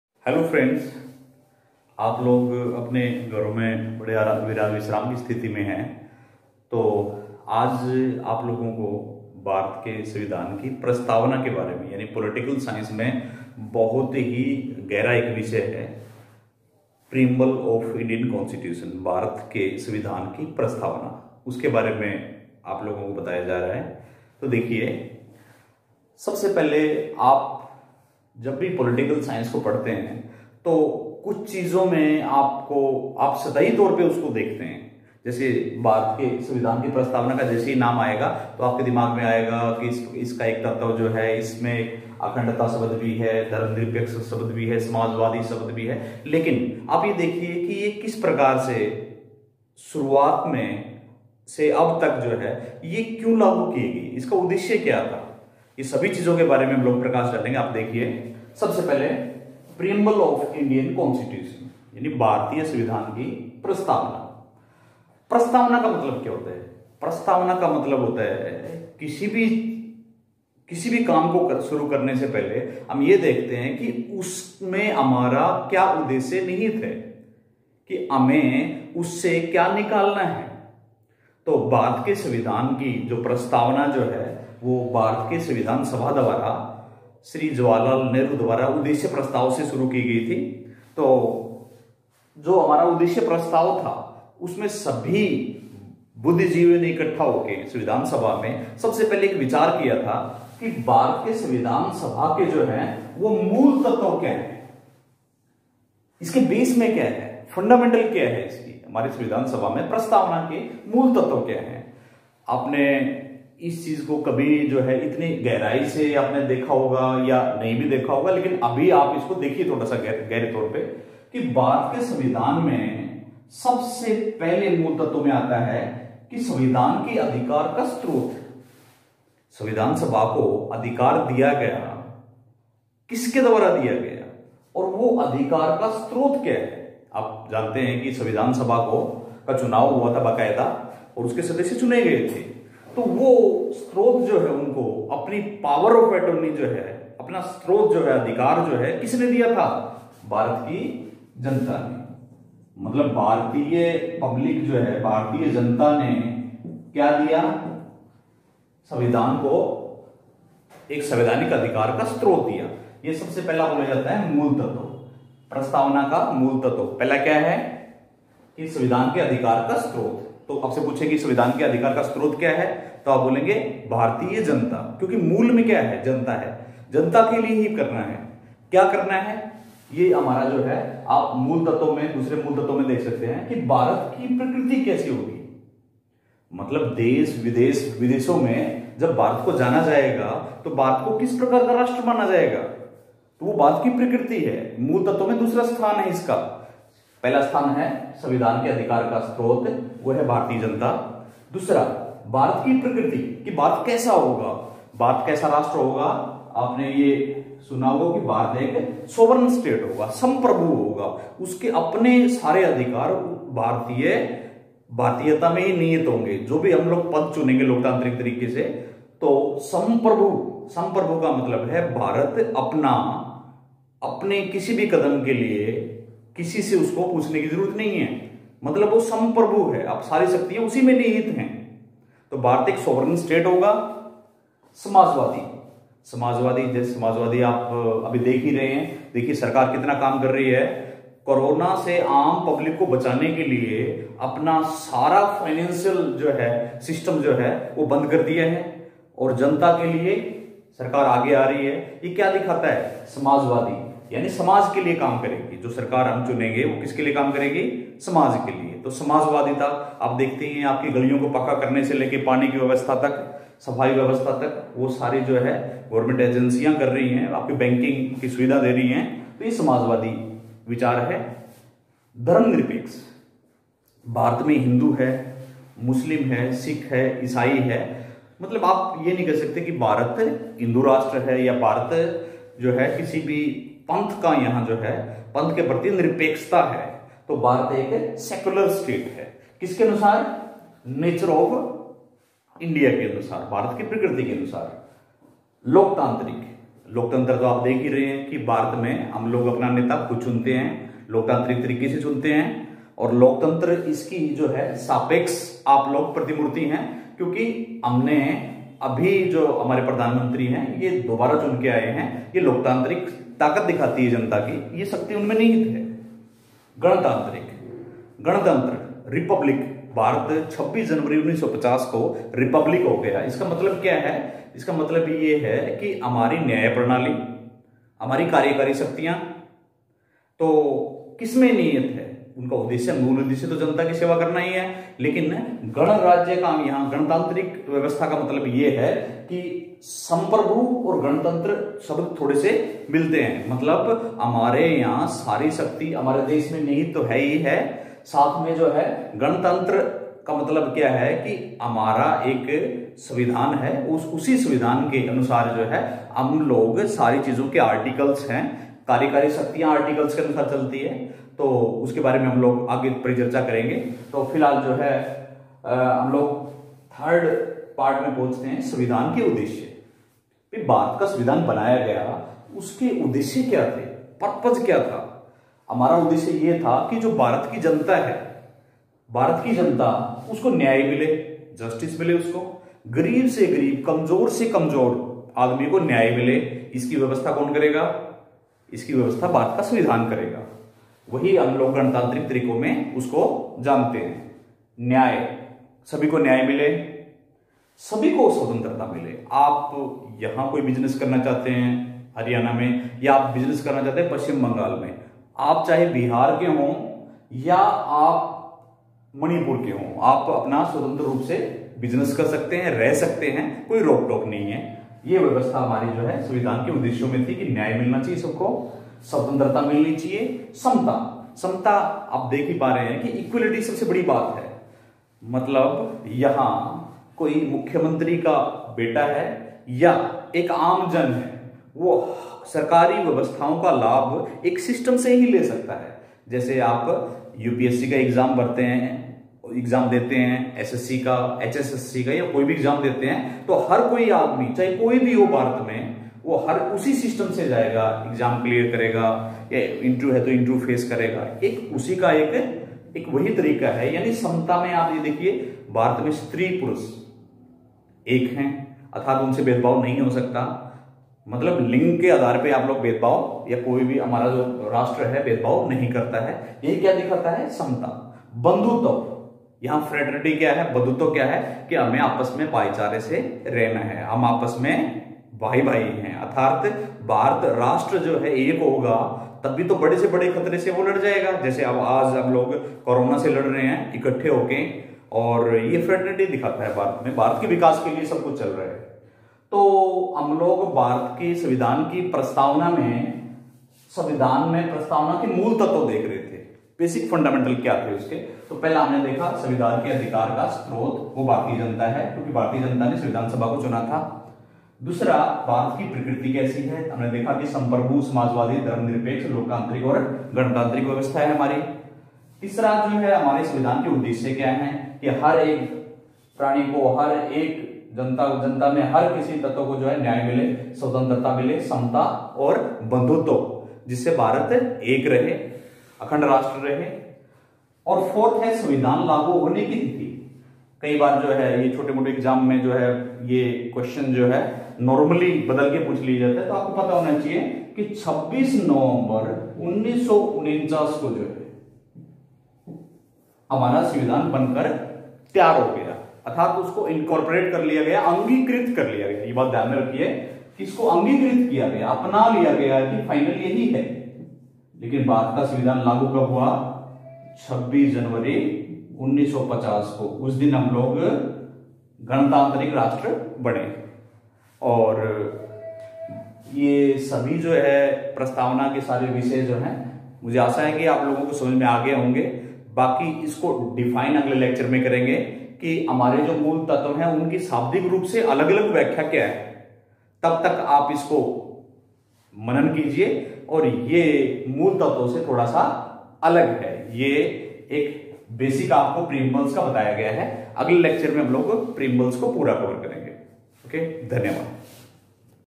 भारतीय संविधान की प्रस्तावना (Preamble of Indian Constitution)
भारतीय संविधान विश्व का सबसे बडा लिखित संविधान है व इस संविधान की एक प्रस्तावना रखी गयी जिसके तहत एक उद्देश्य रखा गया, देखिए संक्षेप मे.